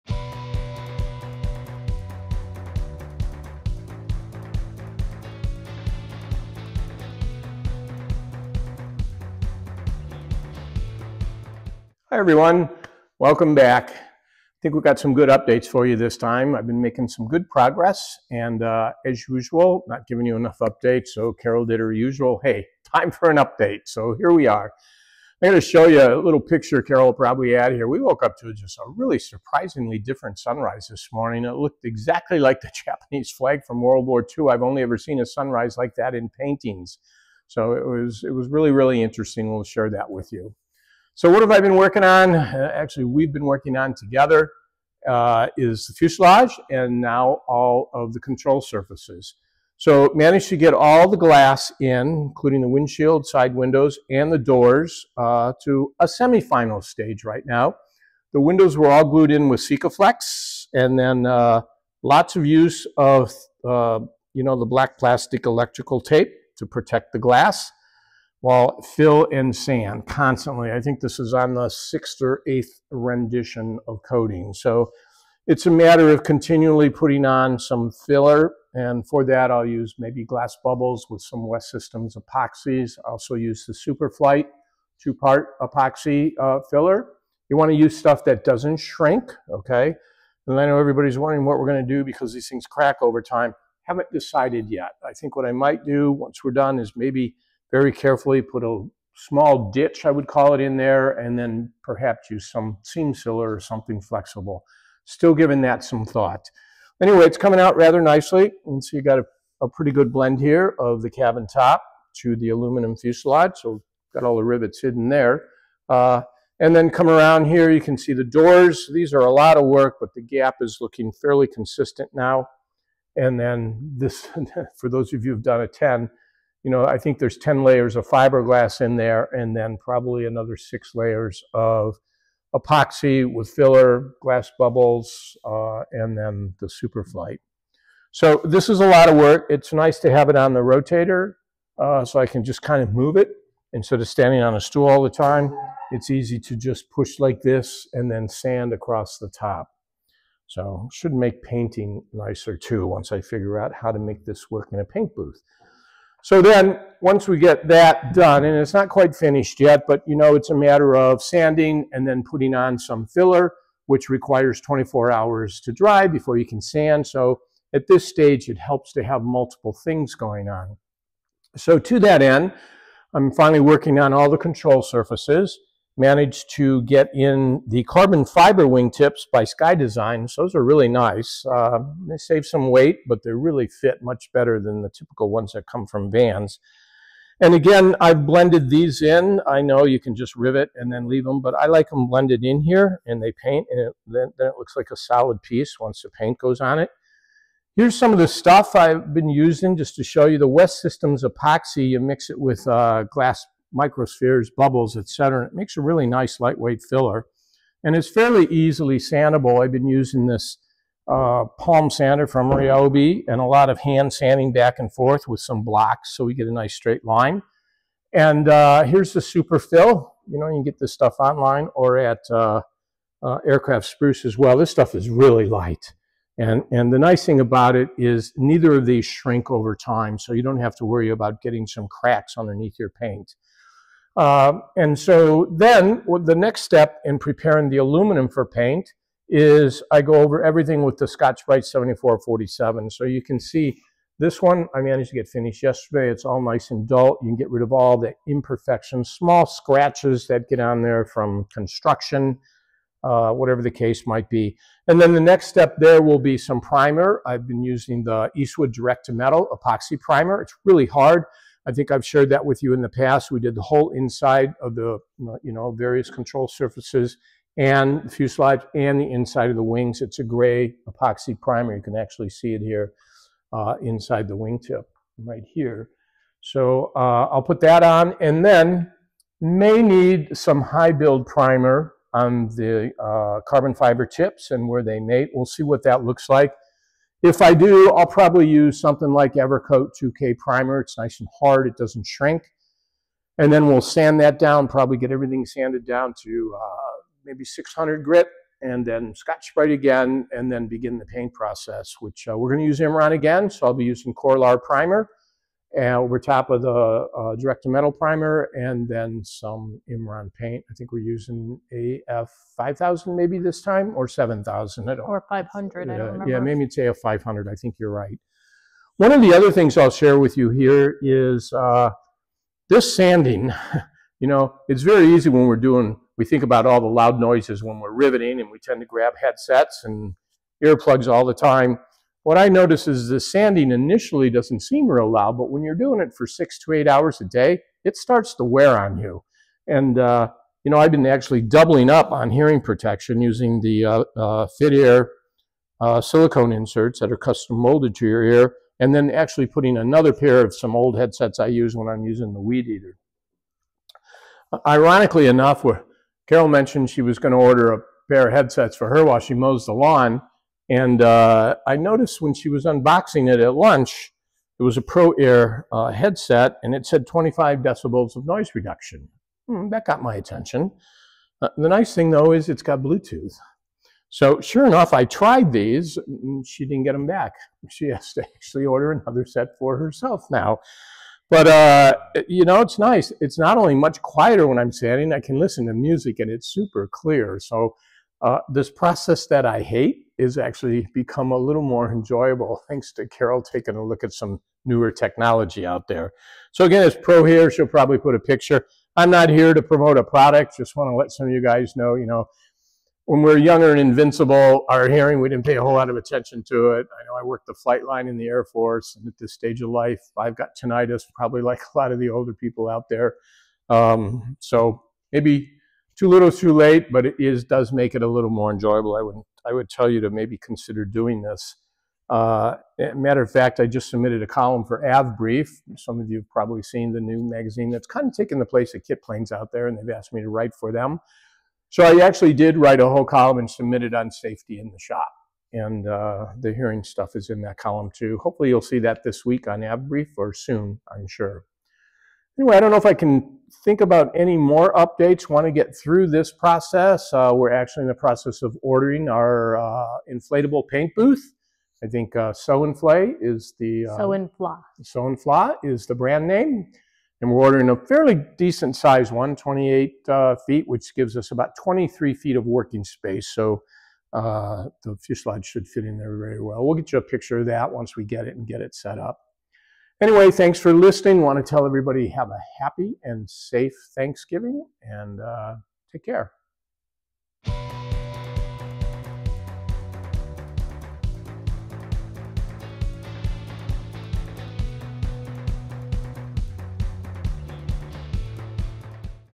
RV 10 build fuselage, control surfaces, sanding and more sanding
Vic from Base Leg Aviation updates us on his RV-10 build process. He shows the windows he has installed on the fuselage and explains the fill and sand process he is using.  He then shows  the horizontal stabilizer with Skydesigns tips.

Products mentioned:
Skydesigns  horizontal tail tip fairing kit

Fasco microspheres:

Prohear 037 bluetooth headset: